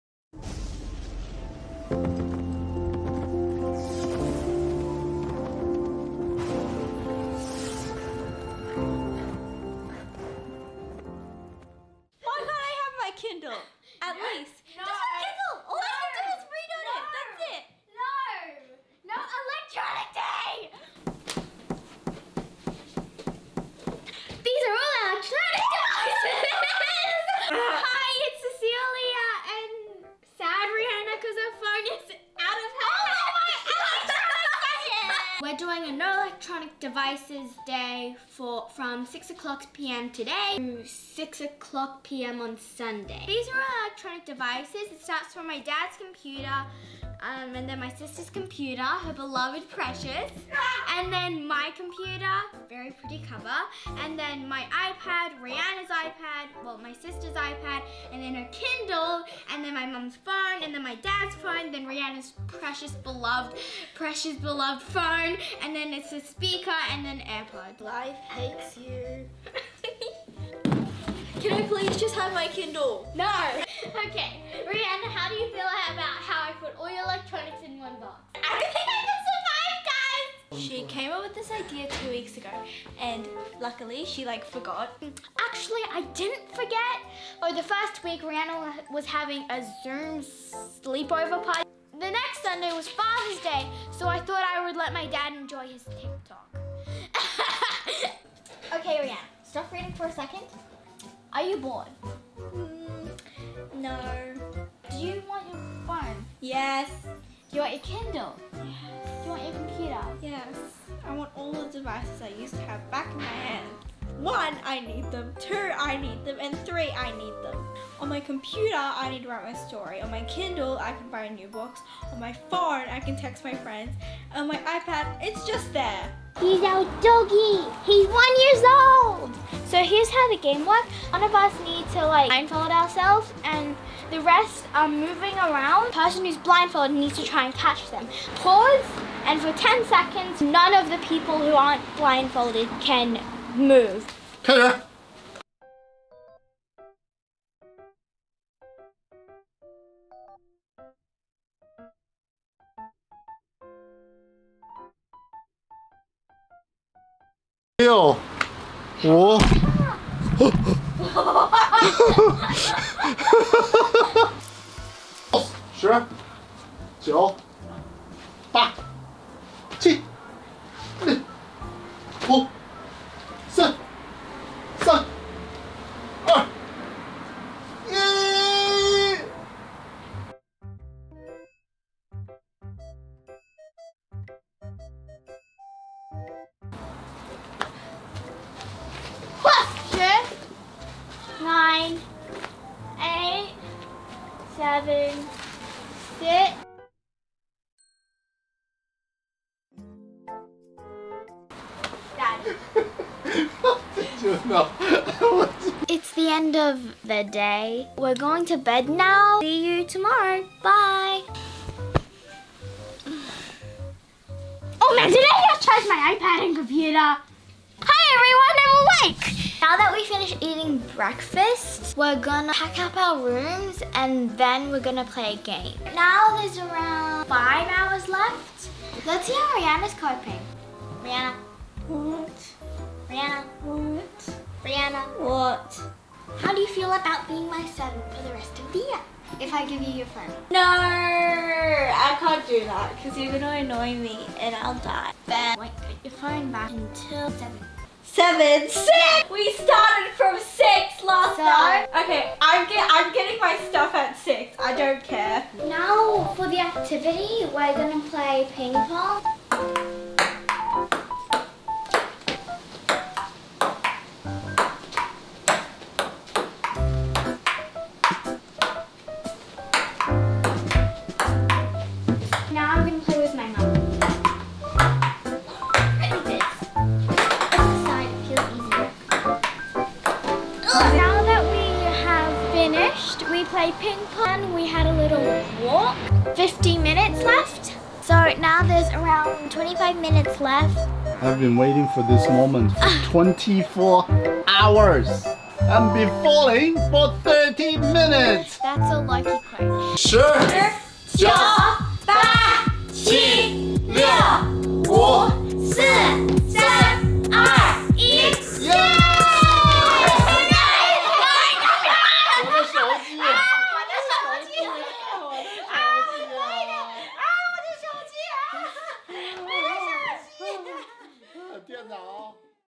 24小时无电子设备挑战-24 hour no electronic challenge
2021年9月12日悉尼封城已经两个半月了，我们只能在家自娱自乐！
Today is 12TH SEP 2021, iT’S BEEN two AND HALF MONTHS
SINCE sYDNEY HAS BEEN in LOCKDOWN,
WE’RE at HOME AND trying to entertain OURFELVES!